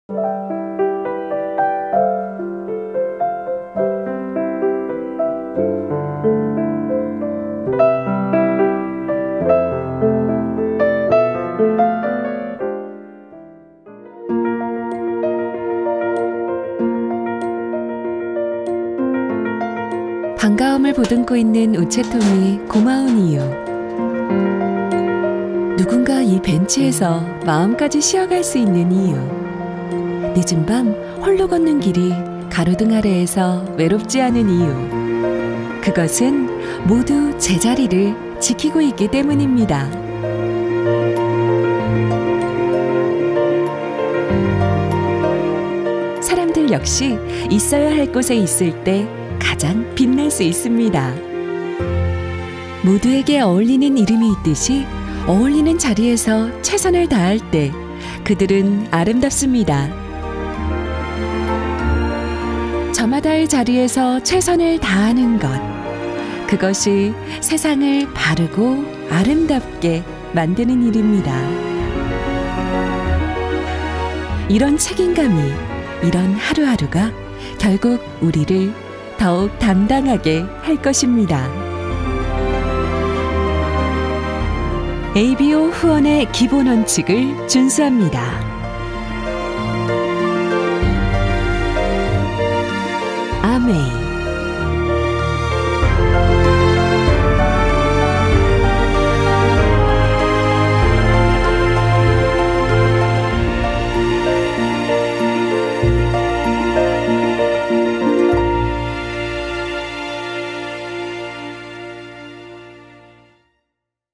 [한국암웨이]Vision 2020 Campaign -ABO등록 유의사항- 편
Vision2020 캠페인은 지속가능한 성장을 위한 한국암웨이와 ABO의 약속을 담은 영상 캠페인 시리즈로 세번째 약속, 편은 올바른 ABO 등록 및 가입 조건에 대한 내용을 담은 영상입니다.